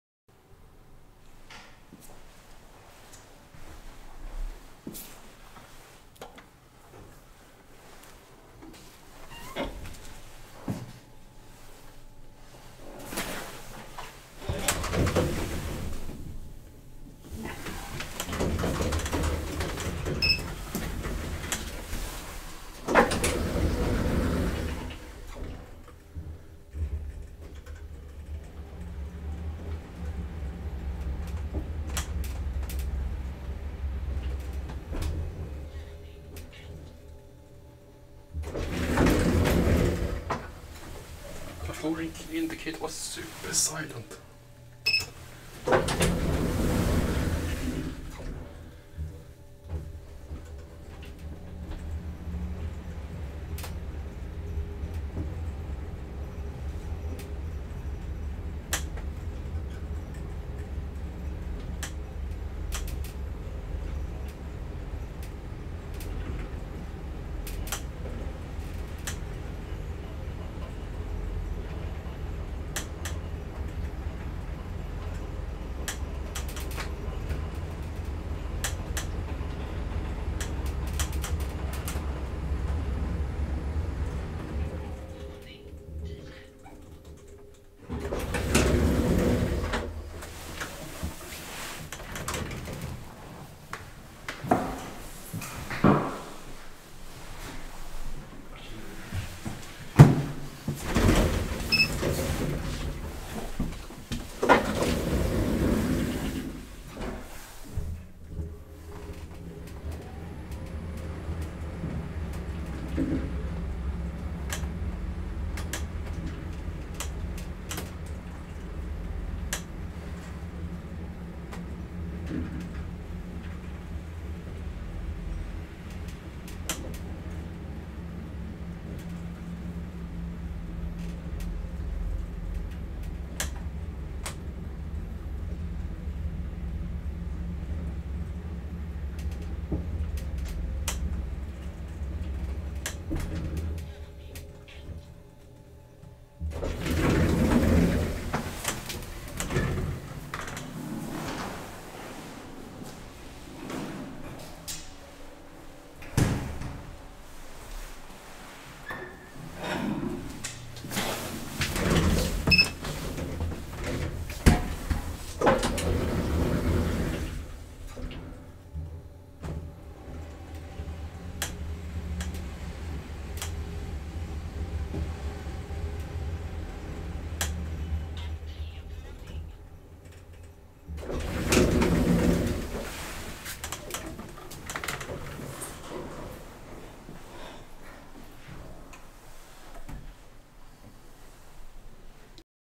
2x Svenska Hiss (Magnusson mod.) Traction Elevators @ Gustavslundsvägen 133, Stockholm, Sweden!
ELEVATOR INFORMATION!

Manufacturer/Tillverkare: Svenska Hiss AB
Model/Modell:
Installation year/Byggår: ~1988
Current modernizer/Senaste modernisering av: Magnussons Hiss AB
Latest modernization year/Senast moderniserat år:
Current model/Nuvarande modell: Hissmekano
Serial Number/Tillverkningsnummer: 88 N 19830 & 88 N 19831
Capacity/Maxvikt: 630Kg - 8 Persons / 1000Kg - 12 Persons
Floors/Antal våningar: 9-10 = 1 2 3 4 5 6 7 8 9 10
Cab refurbished in ~2013 by KONE

Location/Adress: Alviks strand | Hiss I1 & I2 | Gustavslundsvägen 133, 167 51 Bromma (Stockholm/Alvik) SWEDEN!